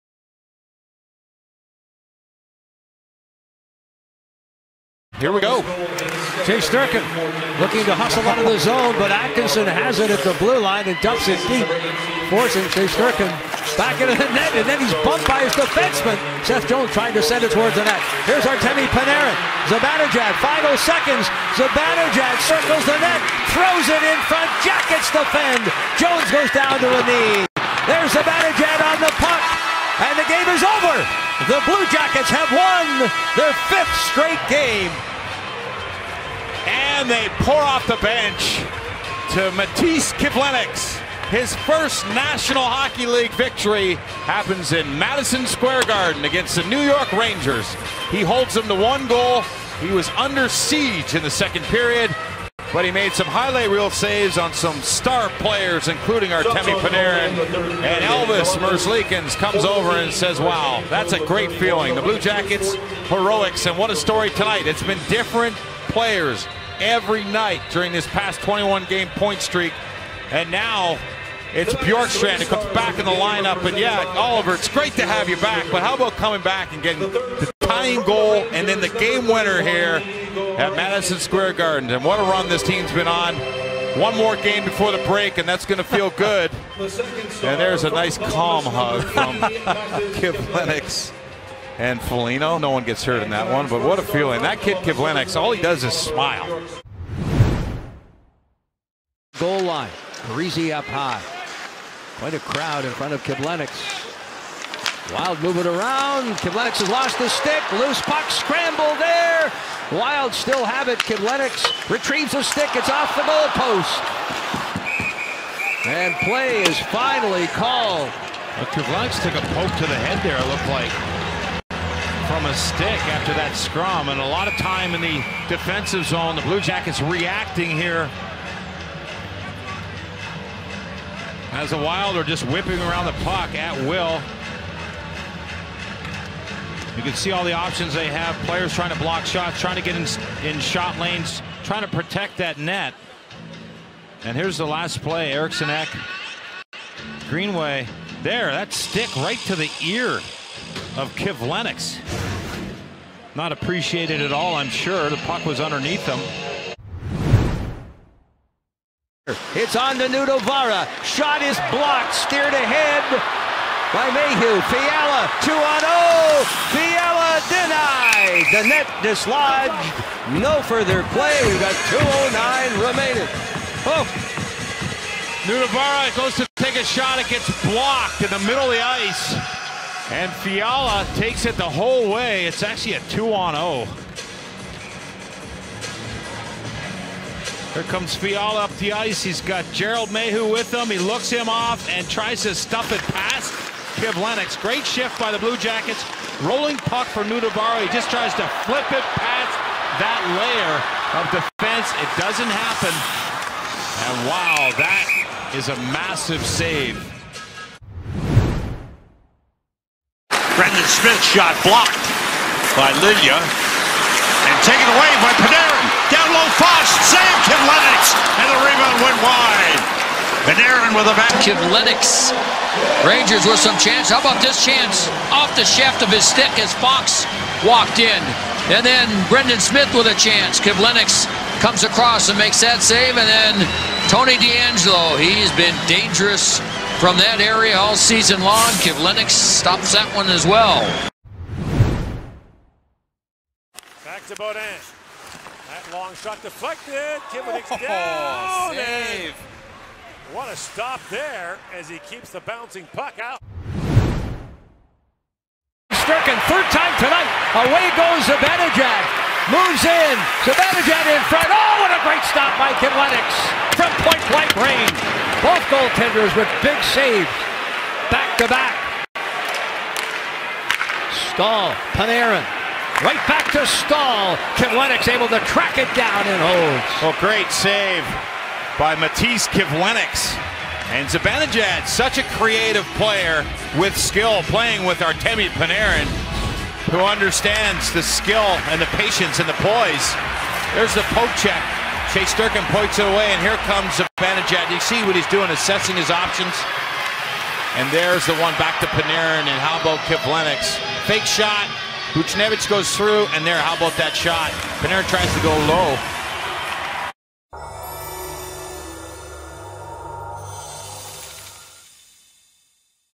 Top 5 Matīss Kivlenieks Saves
After the tragic Death of Matiss, we look at some of his moments in the NHL and with Latvia.